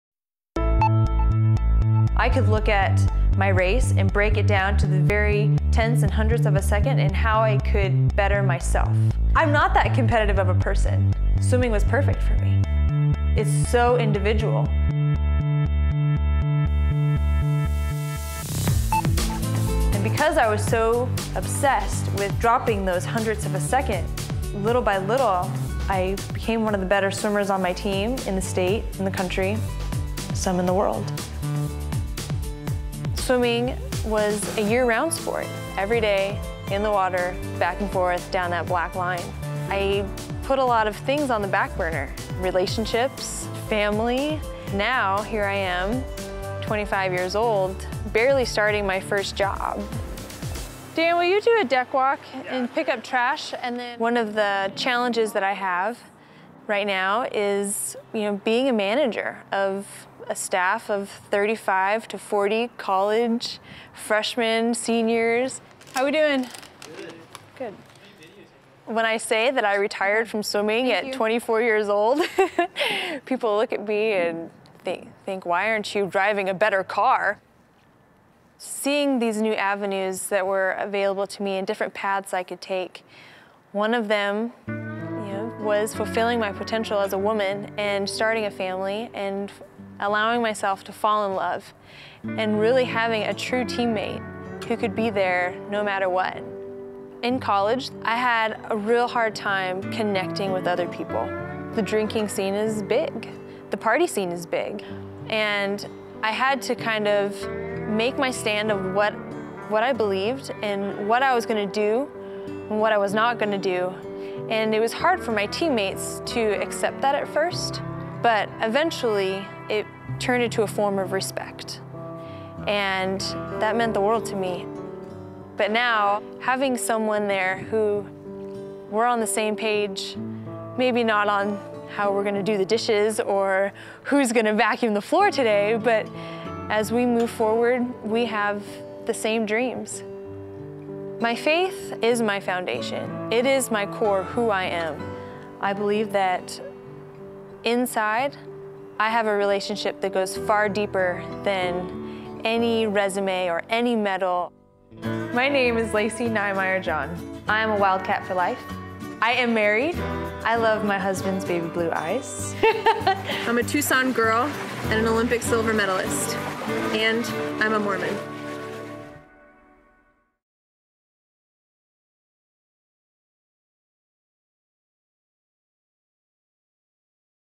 Je suis mormone, médaillée olympique et nageuse à la retraite à 24 ans
Lacey John, médaillée olympique mormone, avait l'obsession d'abaisser ses records de natation jusqu'à ce qu'elle devienne la meilleure. Aujourd'hui, elle découvre d'autres domaines dans lesquels progresser.